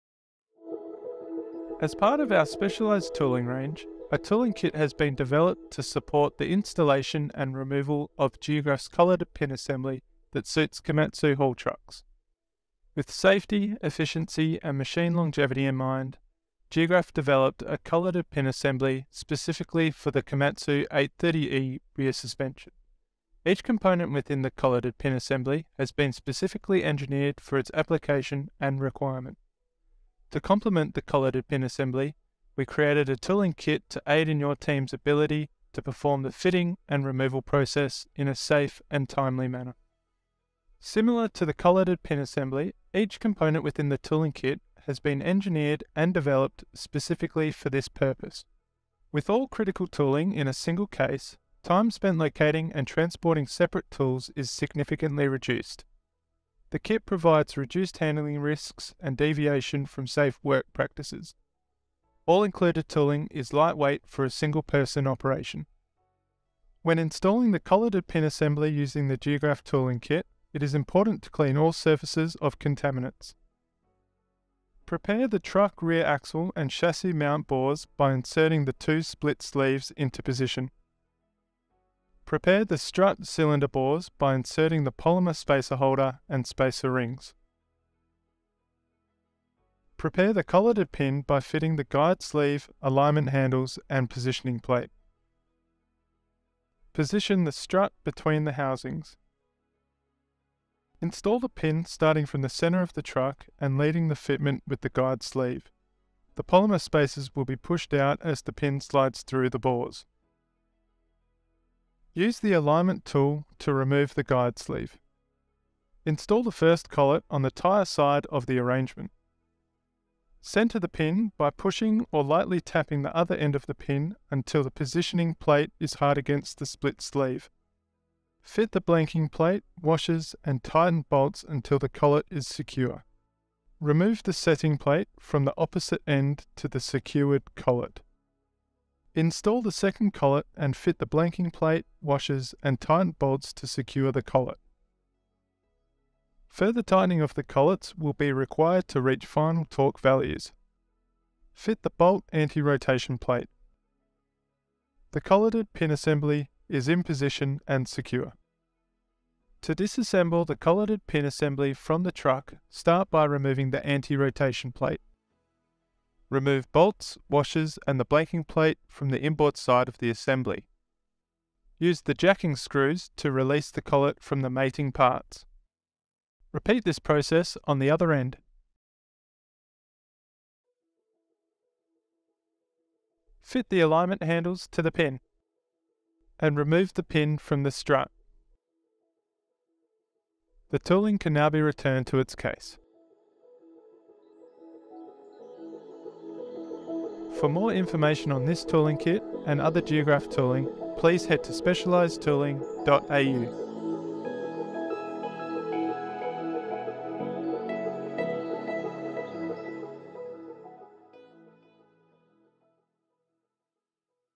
Colleted pin removal tooling to suit Komatsu 830E haul trucks. (Removal of thermal lancing).
Geographe has created a colleted pin assembly to suit Komatsu 830E haul trucks to eliminate the issue of thermal lancing on standard pins in the bores.

Unique to the Komatsu 830E haul truck, Geographe has developed a tooling kit to aid in the colleted pin assembly and disassembly process.